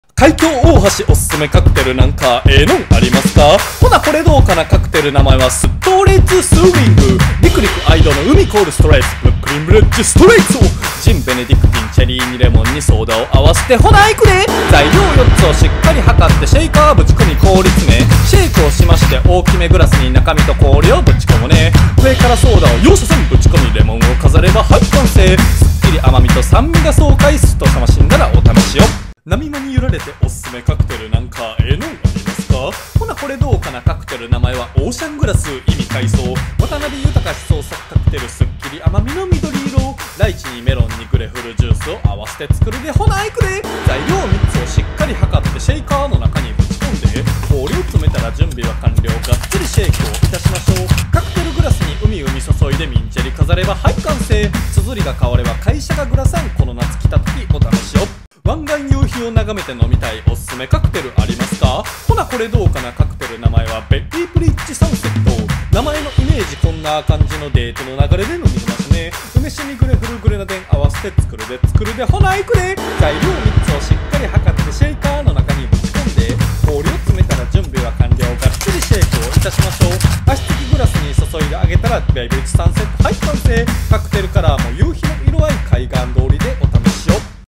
海沿いなかんじのカクテルを3つ選んでみた
今回のカクテル
①ストレイツスリング
②オーシャングラス
③ベイブリッジサンセット

★がんみのカクテル小学校がつくってるカクテルサイト
Bars Beginner

⚠️お酒は20歳になってからよ